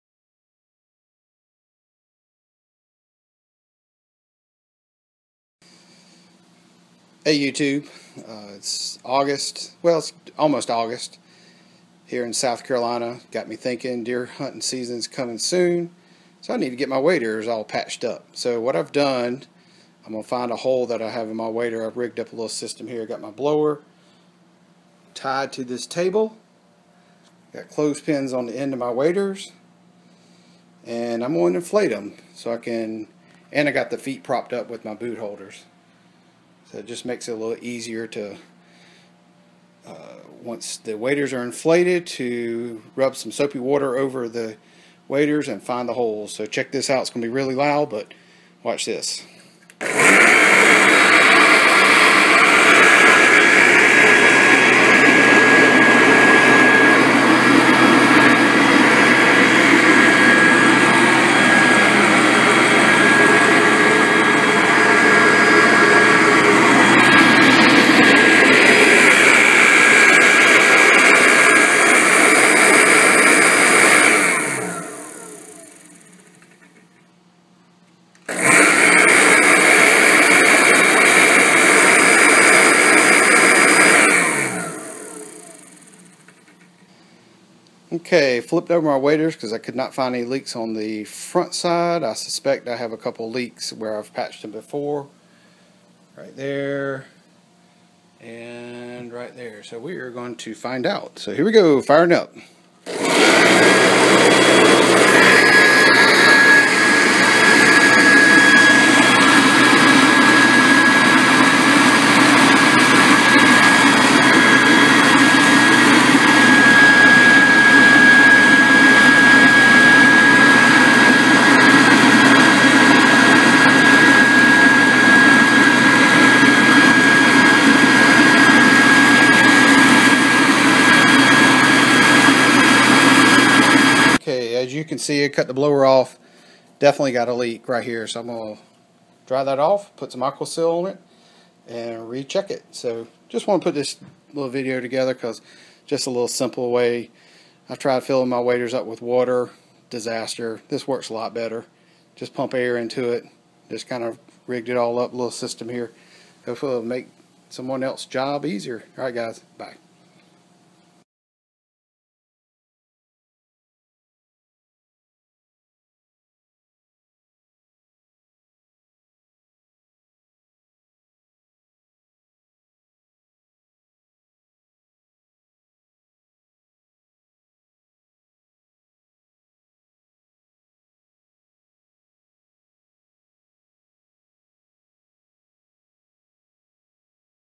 How to find holes in neoprene waders with a blower
Easy way to find holes in neoprene waders using a leaf blower and soapy water.